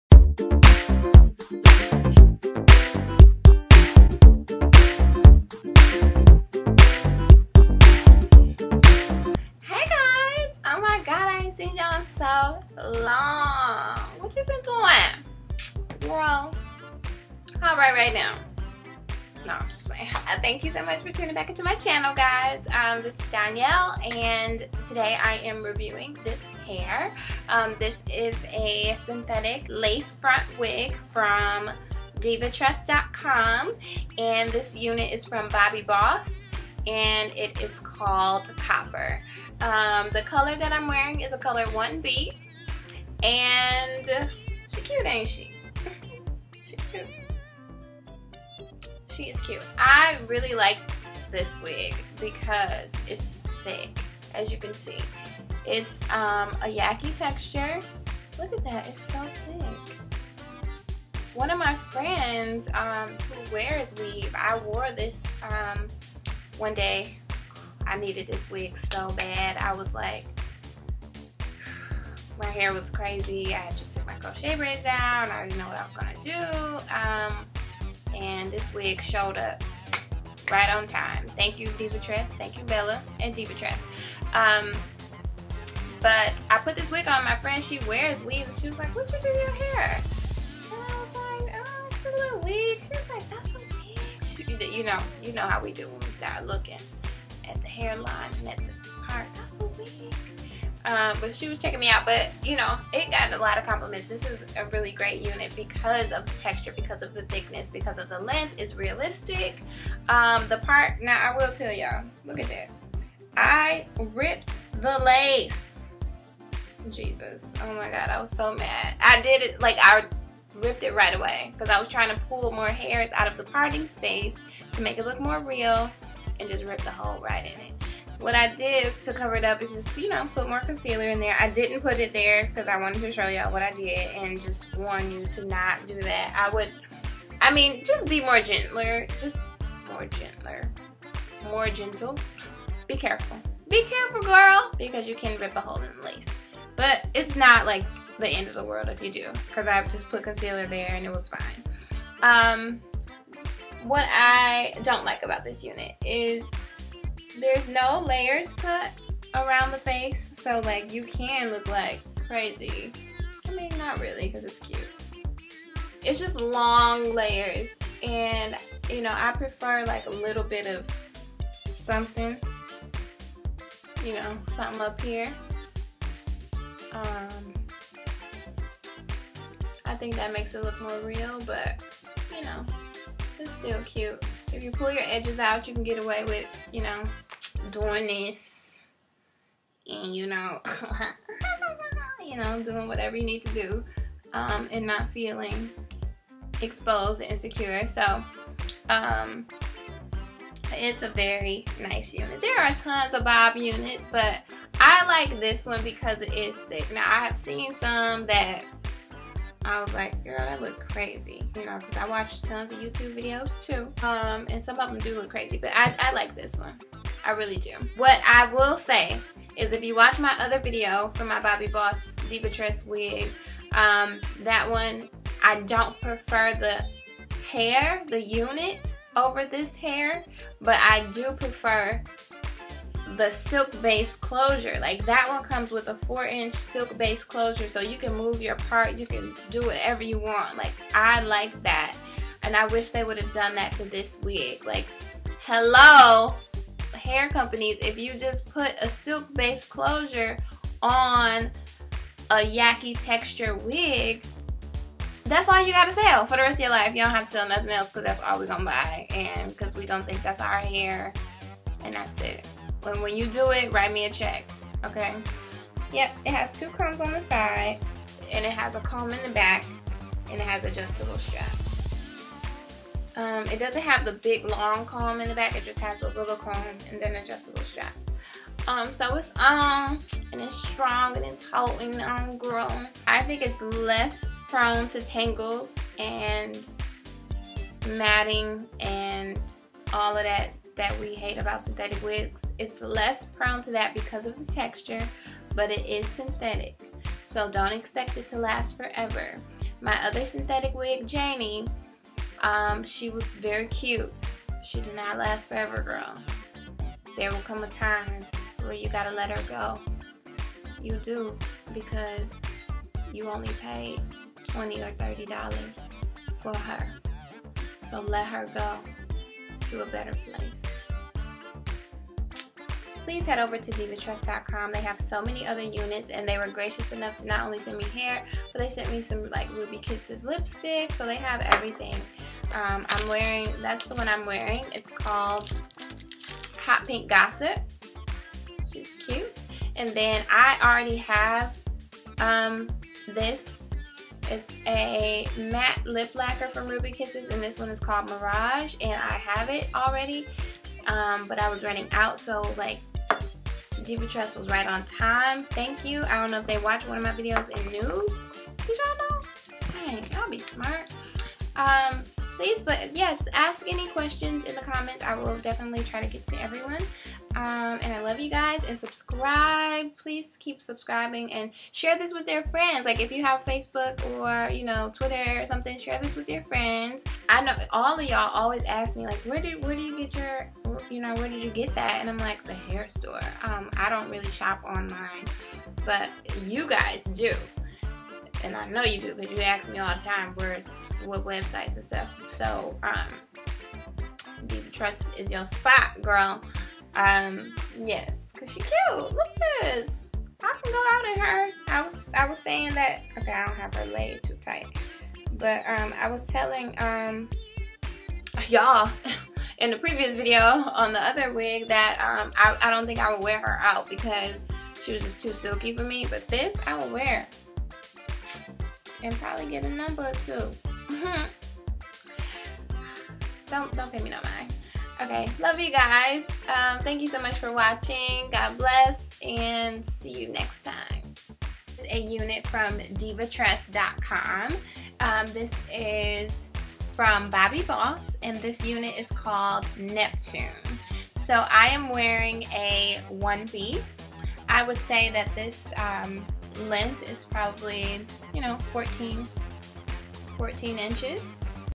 Divatress.com | Bobbi Boss Synthetic Lacefront Wig - Copper | Review
Divatress.com was lovely enough to send me this unit from Bobbi Boss, Style Name - Copper.

BOBBI BOSS LACE FRONT WIG – MLF-74 COPPER Color 1B